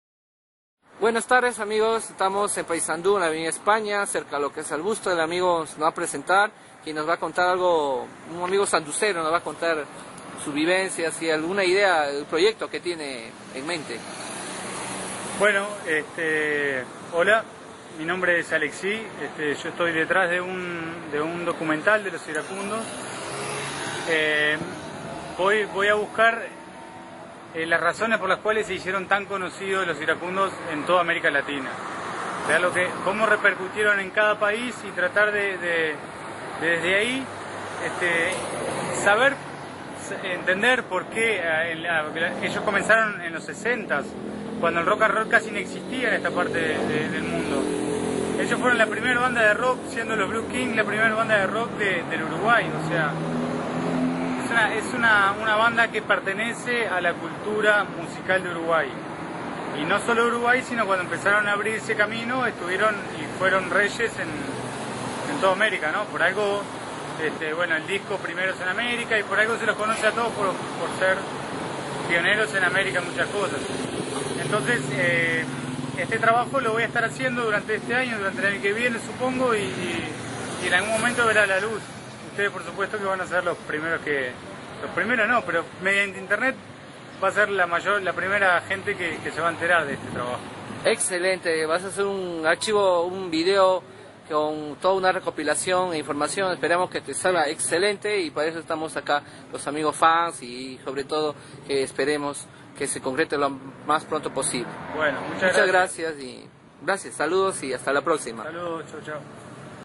Con Alexis Bourgeois fan sanducero de LOS IRACUNDOS
El fan  Alexis Bourgeois esta en proyecto de  realizar un documental de LOS IRACUNDOS.